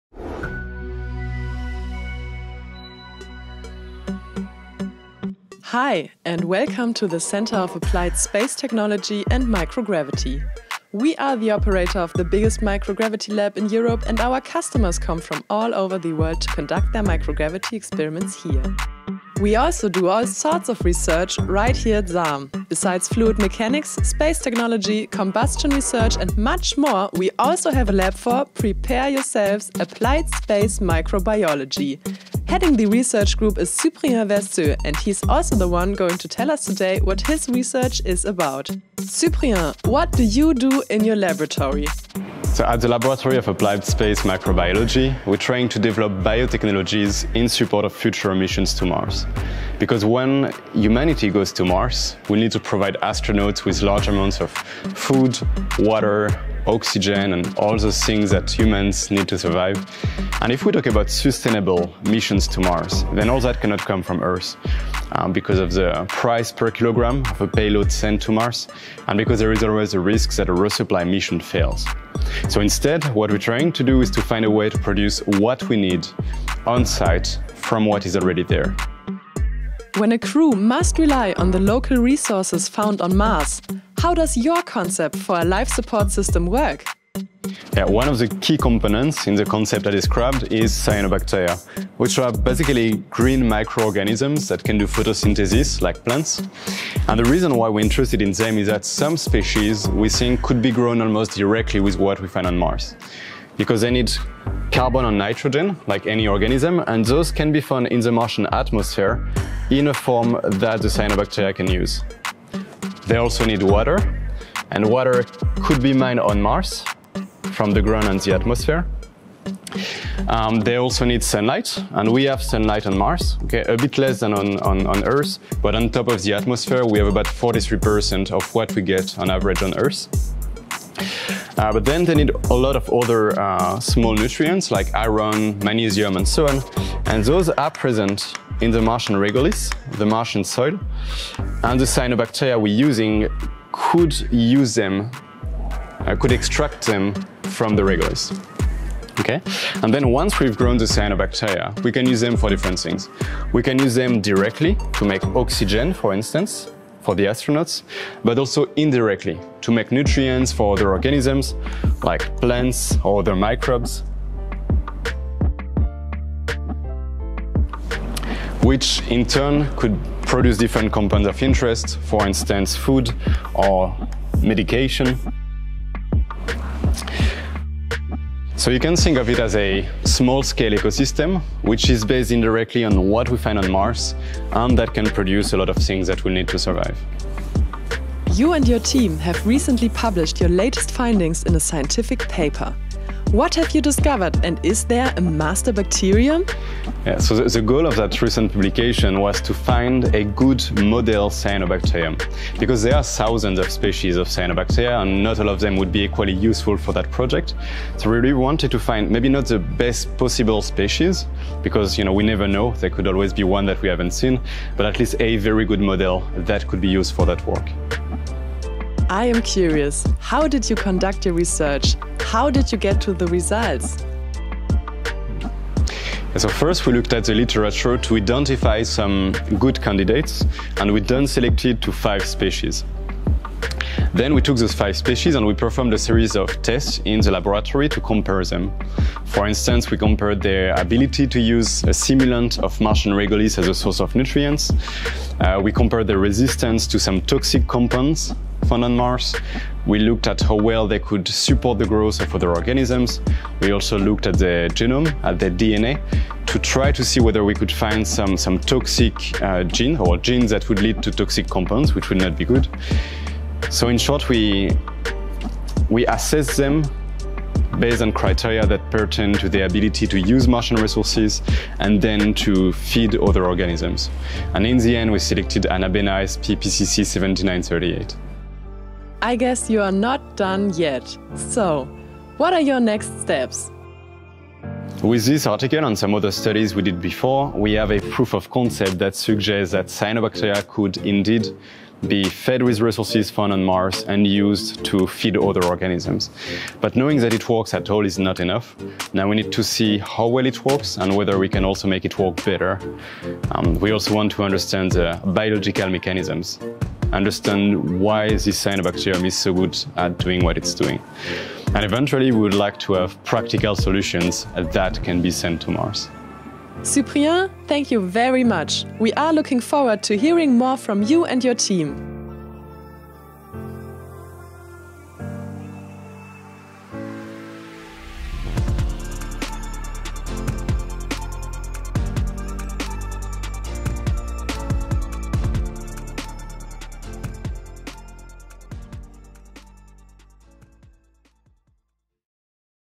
Mars dust as a basis for life? - Laboratory of Applied Space Microbiology
More than 10 scientific research groups are located at ZARM, the Center for Applied Space Technology and Microgravity in Bremen. Cyprien Verseux is heading the ZARM laboratory of applied space microbiology (LASM). The LASM was created, primarily, to develop microbe-based technologies in support of future crewed outposts on Mars. The star in his research is a microbe called cyanobacteria. Find out why this microbe, particular a strain called Anabaena sp. PCC 7938 is so interesting to Cyprien and his team.